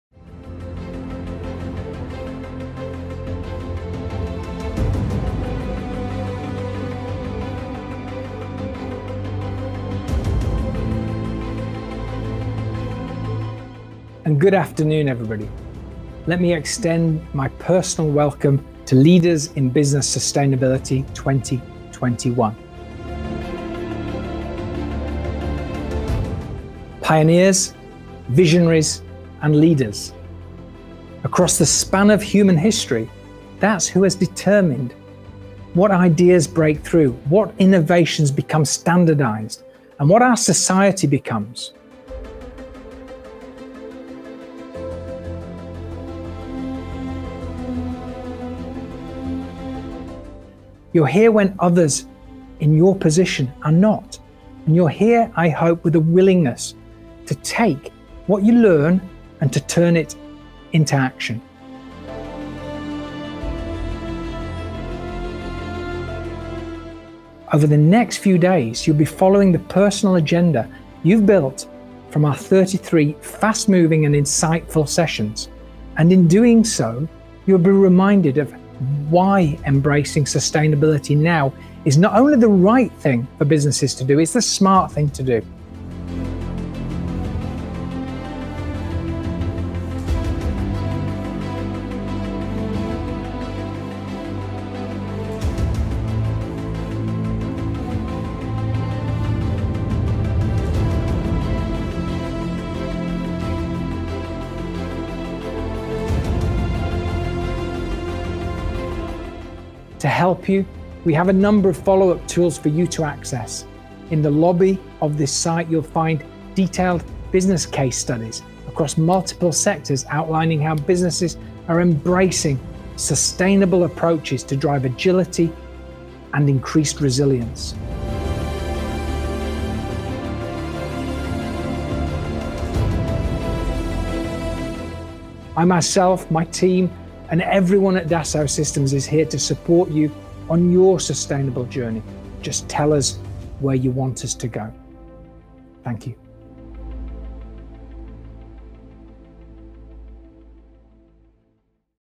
Dassault Systèmes - Case Study (June 2021)
Crystal Interactive produce a leadership conference for Dassault Systèmes - 3-day event, with 7-acts and 55 speakers.

Sian Jones facilitates with Crystal delivering the Virtual Event Platform and flexible green-screen studio.